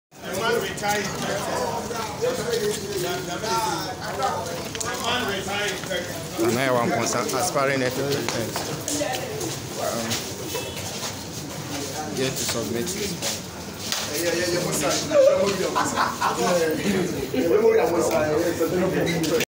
Phar Rangers President submits his forms
Nana Yaw Amponsah files his nomination forms for GFA presidency at the secretariat